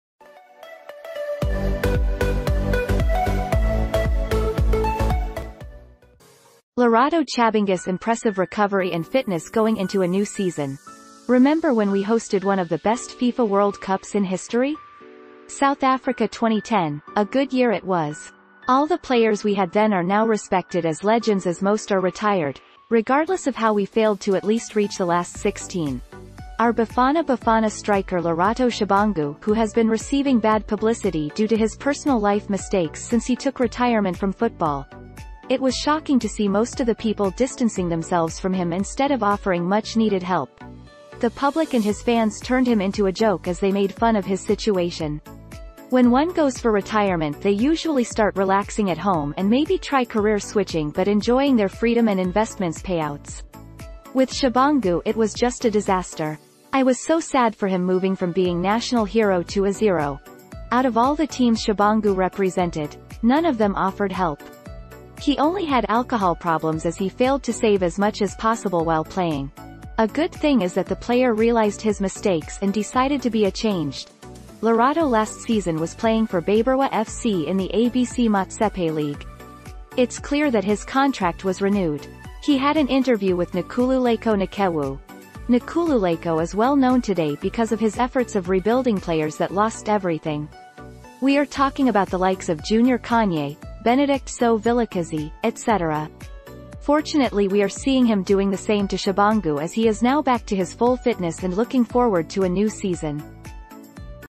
Lerato Chabangu’s impressive recovery and fitness going into a new season
Lerato Chabangu’s impressive recovery and fitness going into a new season - Remember when we hosted one of the best FIFA world cups in history? South Africa 2010, a good year it was. All the players we had then are now respected as legends as most are retired, regardless of how we failed to at least reach the last 16.

Our Bafana Bafana striker Lerato Chabangu who has been recieving bad publicity due to his personal life mistakes since he took retirement from football.

It was shocking to see most of the people distancing themselves from him instead of offering much needed help.
▬▬▬▬▬▬ஜ۩۞۩ஜ▬▬▬▬▬▬
🎥 MAY YOU WILL LIKE:

▬▬▬▬▬▬ஜ۩۞۩ஜ▬▬▬▬▬▬
🎥 MORE VIDEO BIOGRAPHY SOUTH AFRICA: